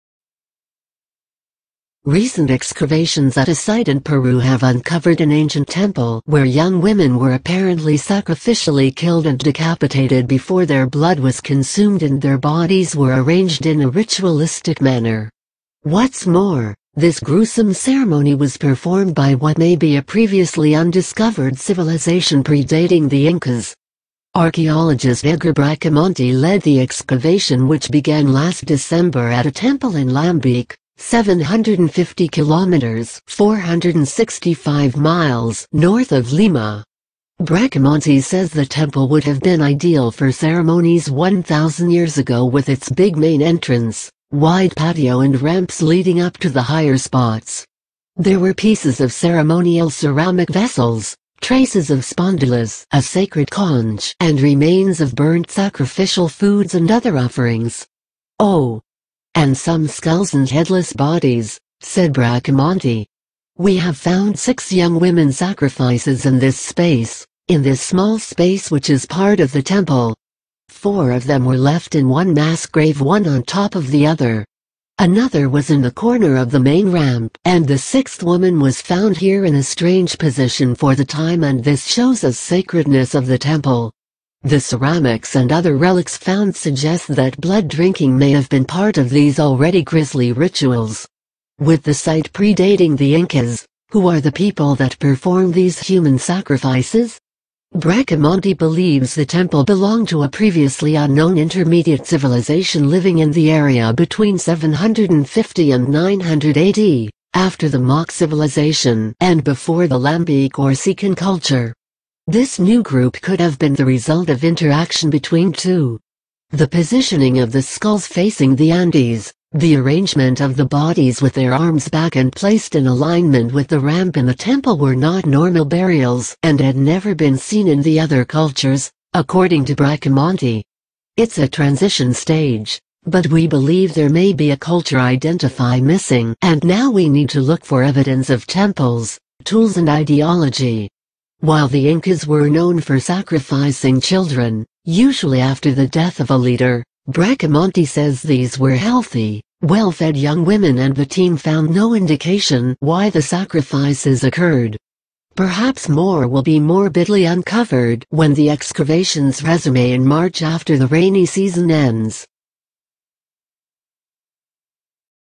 1,000 Year Old Remains of Human Sacrifices Found in Peru
1,000 Year Old Remains of Human Sacrifices Found in Peru